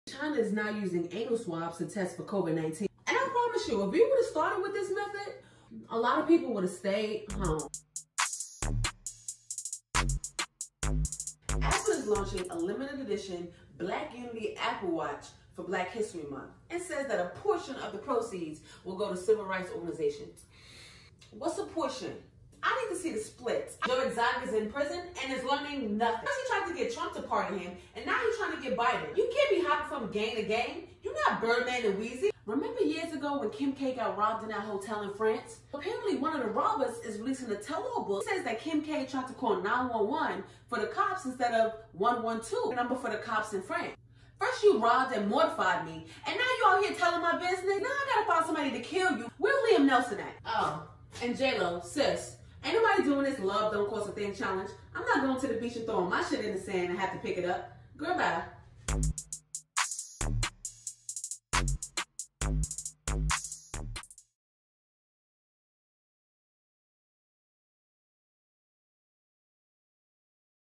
A WEEK IN 60 SECONDS WITH LAH [EP 40] (1/31/2021)
Apple bringing us Black Unity timepieces, Joe Exotic is gang gang, Kim K should hire Liam Neeson, Love do be costing a thing actually…… and more